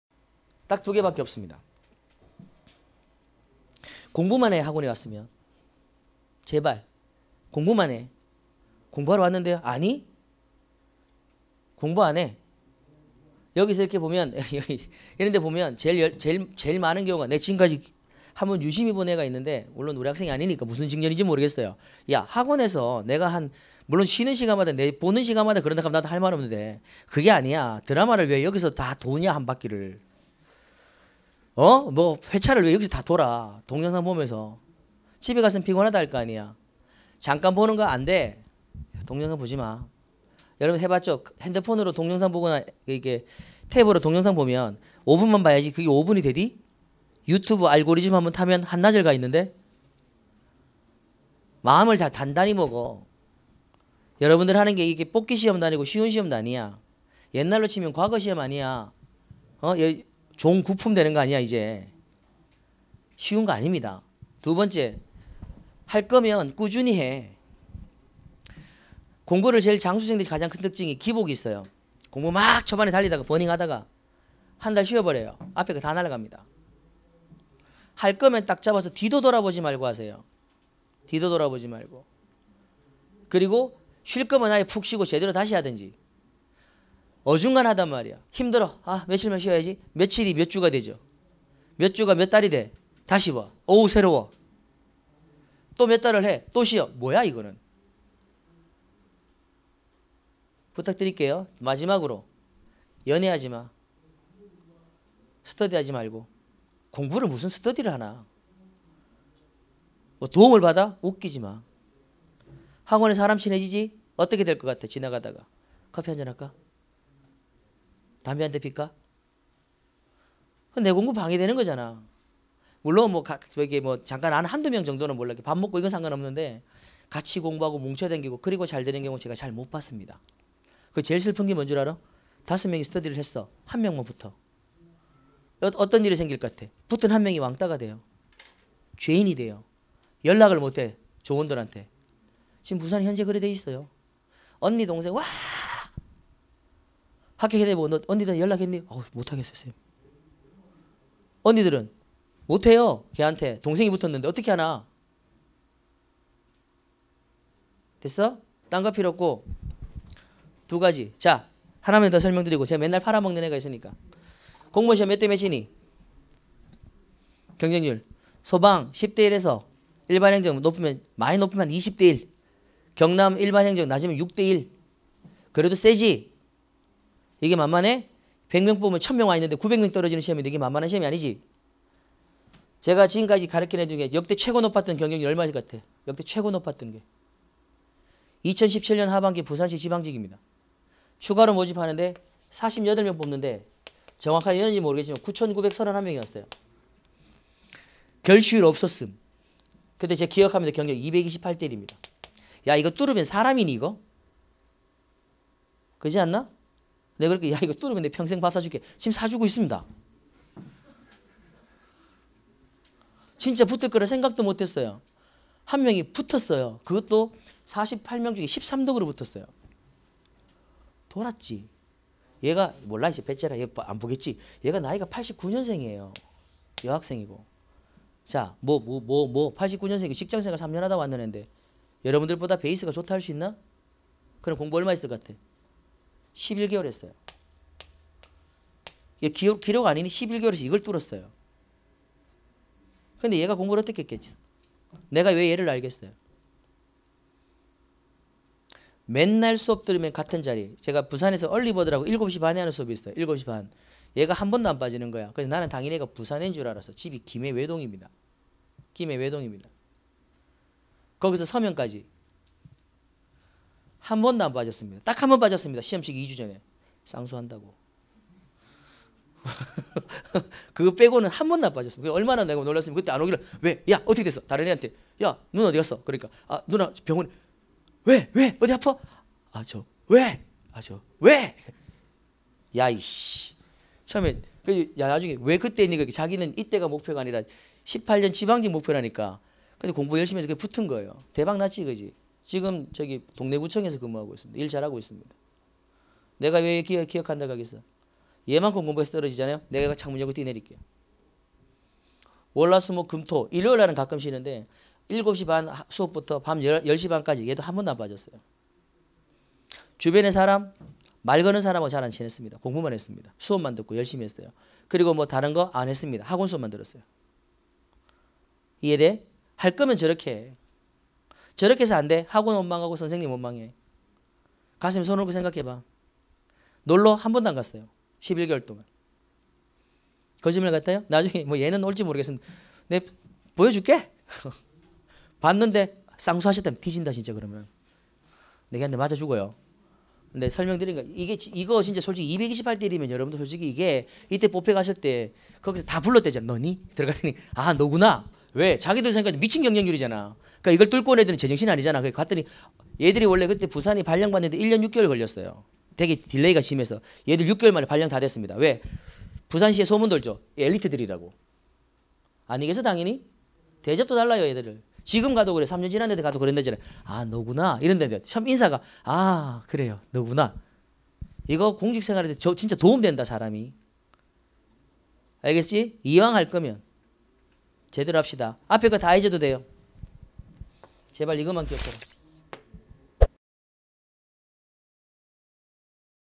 공무원영어, 공부 할거면 제대로 해! | 공무원영어 합격방법
시험을 준비하는 여러분 지금 공부 잘 하고 계신가요?
그럼 지금 보는 영상은 혹시. 몇번째?

공부할때 연애, 스터디 고민 김재운 선생님이 해결해 드립니다.

친절한 재운쌤이 보여주는 많은 수험생의 모습 같이 보실래요?

영어를 잡아야 합격을 잡습니다.
9급공무원영어는 캐칭영어 김재운